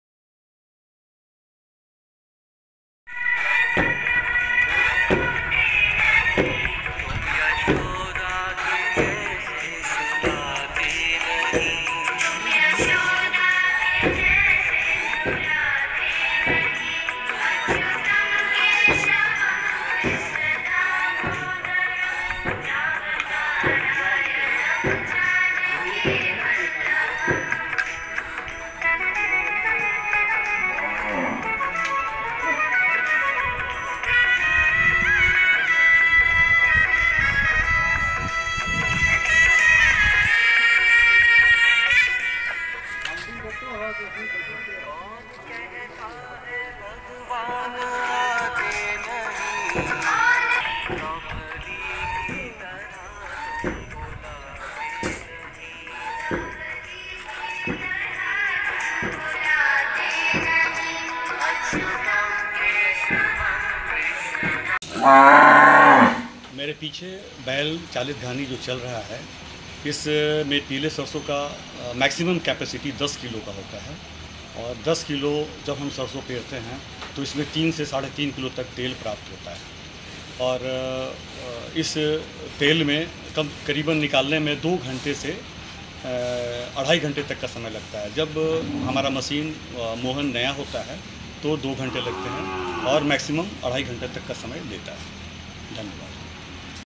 Vakshi Video 112- Vakshi's Bull Driven Wooden Pressed Oil Source - Adya Organics
Know in this video where does Vakshi's Bull Driven Wooden Pressed Oil Come from .

Be wise
Go Organic...Go Healthy
with
Vakshi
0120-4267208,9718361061
Spna smridh bharat ka _/\_